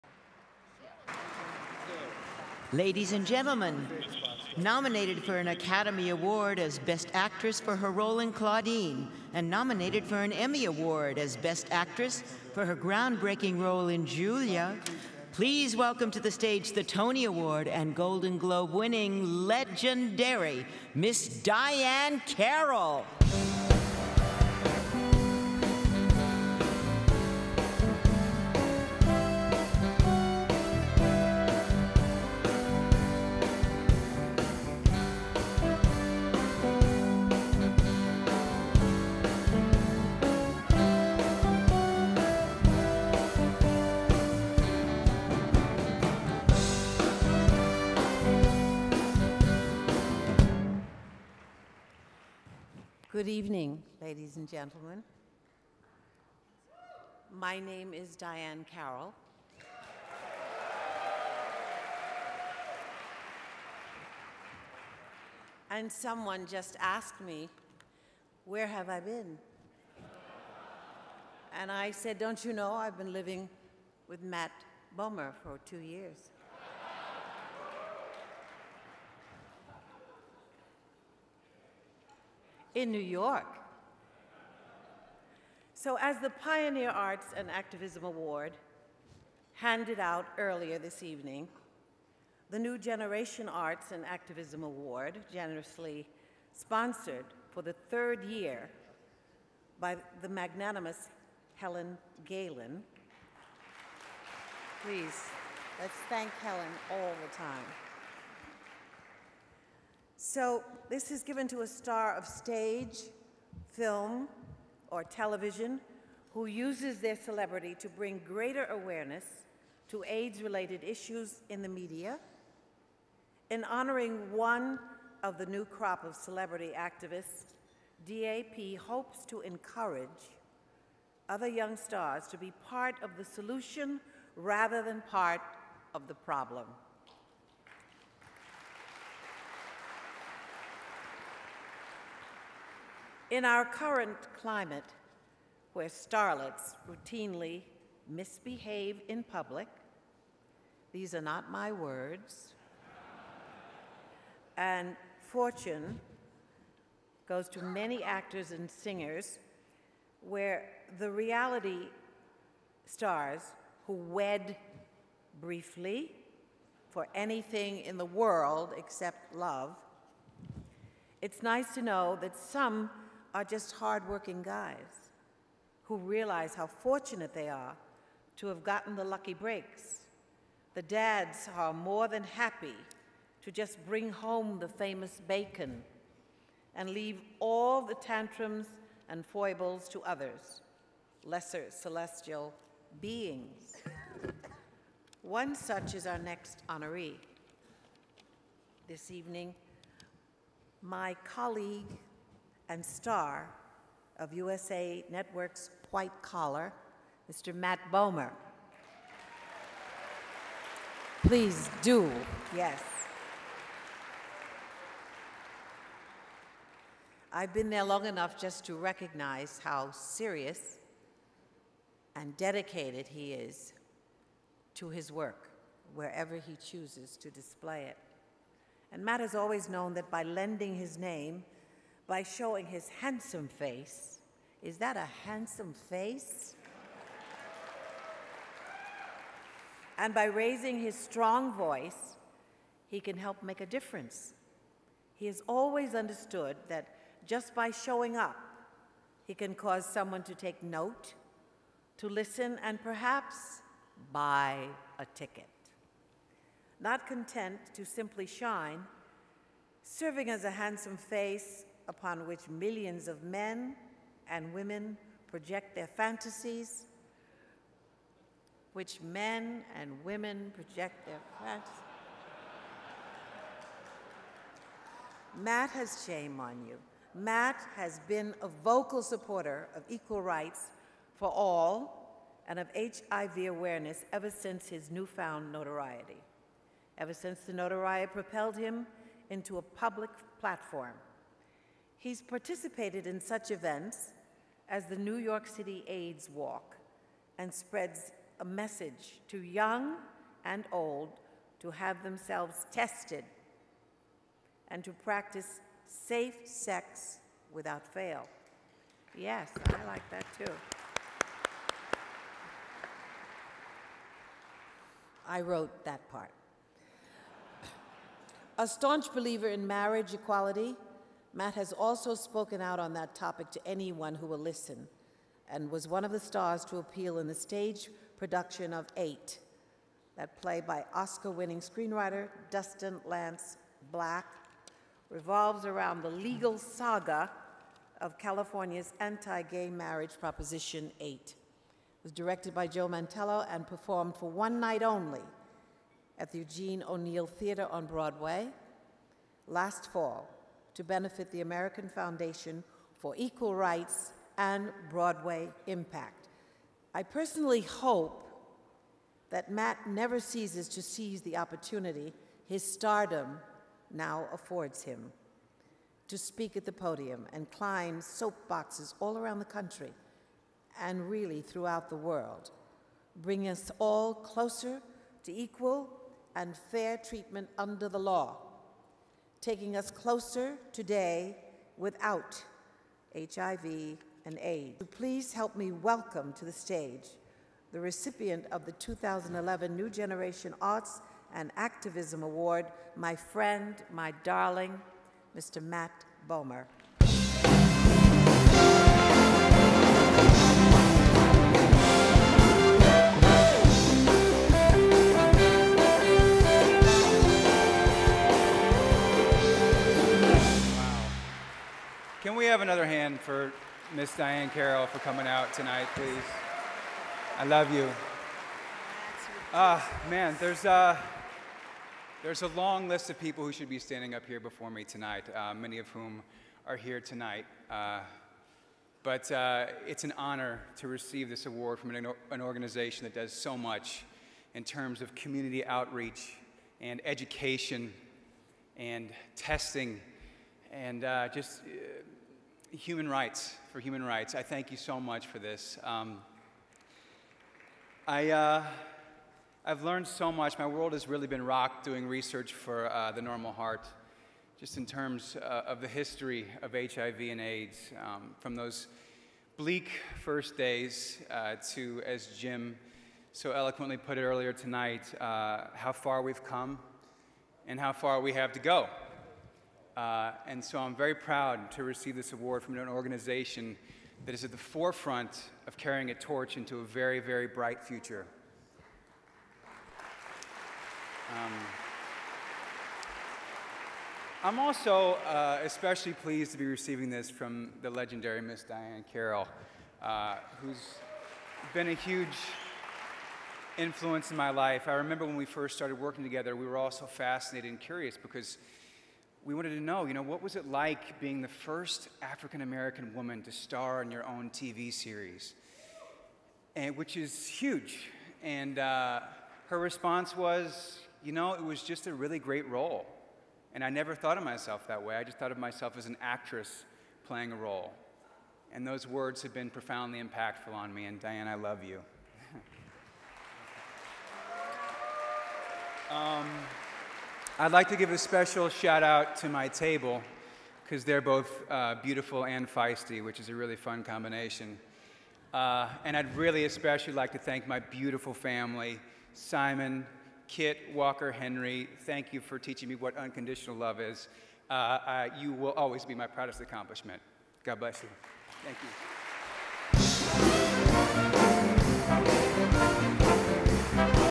Diahann Carroll introduces Matt Bomer
[Diahann Carroll appears on screen at 00:41] The legendary Diahann Carroll introduces her esteemed colleague, Mr. Matt Bomer from the USA Network show, "White Collar" to receive the 2012 New Generation Arts & Activism Award